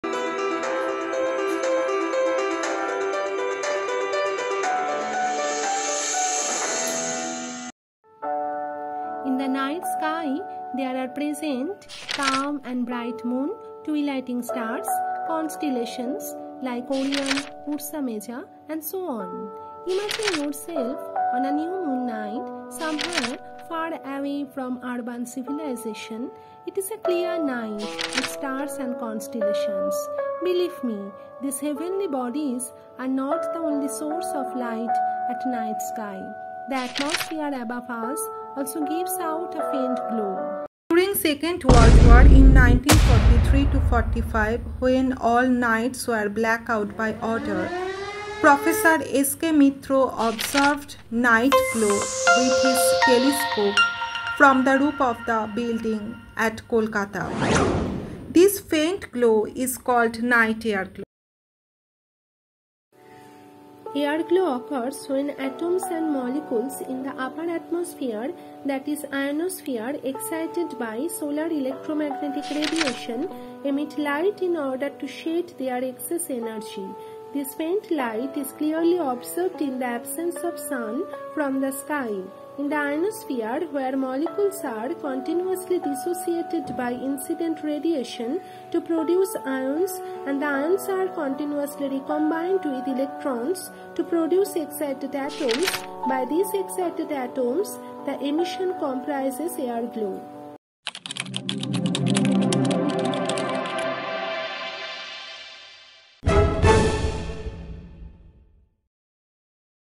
Mystery of Night Sky Luminescence
Sun, moon, and all celestial bodies are not the only source of light. There is present other source . what are they? In this discovery , Prof S K Mitra's contribution is remarkable . His real story helps to reveal this faint glow.  Mystery  of the Universe still unrevealed.